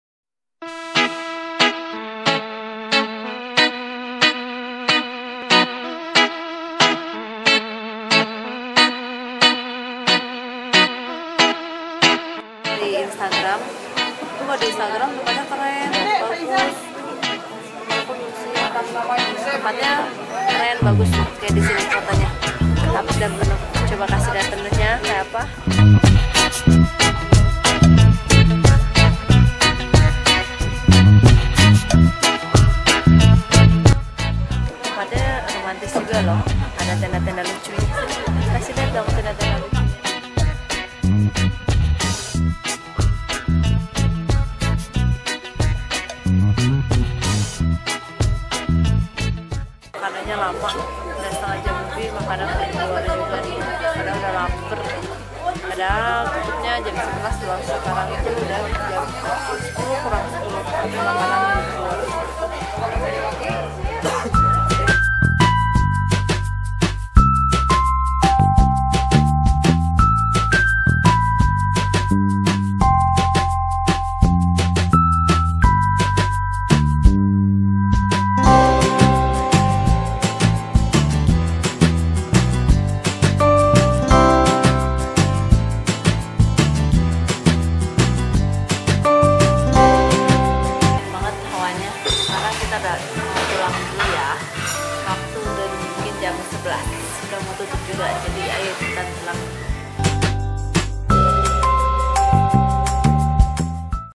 Holiday Bandung 2016 (PART 2) Lereng Anteng - Punclut
Bandung gada abis-abisnya di boomingkan dengan tempat baru kali ini di daerah Punclut. namanya Lereng Anteng dan langsung menjadi hunting spot bagi para pencari spot foto seperti foodie, instagrammer. enjoy my video..